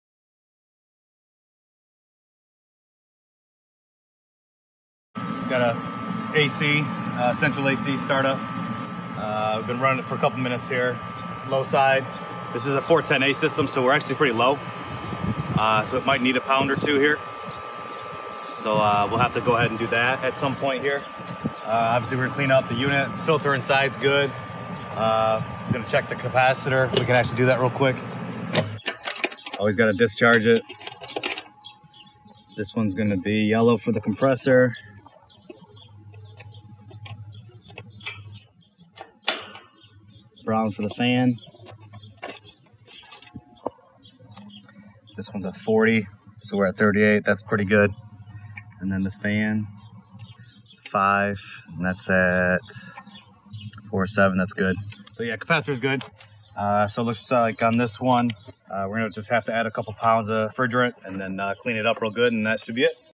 Central Air Conditioner - R410A refrigerant Charging - Diagnostic & Repair
puron
mo99
407c
r407c
r-22
r22
410
410A
R410a

This video is a ProTech repair, filmed by the technician in the customer's home.

Repair a damaged appliance that no longer works.

We take you into a customer’s actual home and teach you how to "Fix it Yourself and Do it Right the First Time"